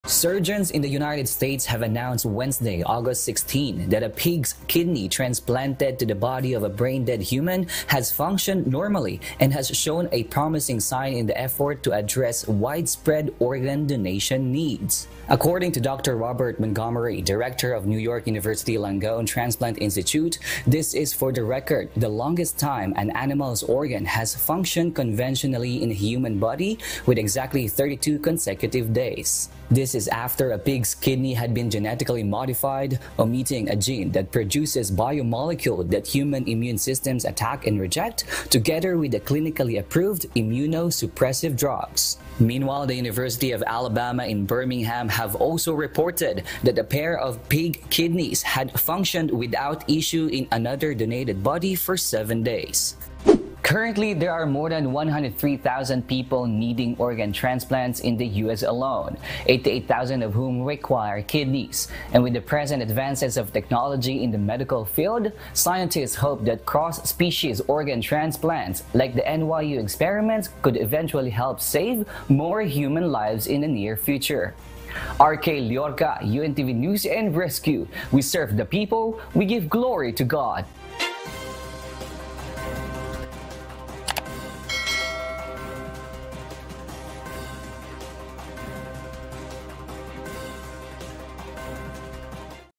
U.S. scientists announce longest working pig’s kidney transplant to human
Scientists in the United States celebrate after a pig’s kidney that was transplanted to a human showed a milestone as it continues to function normally for a month.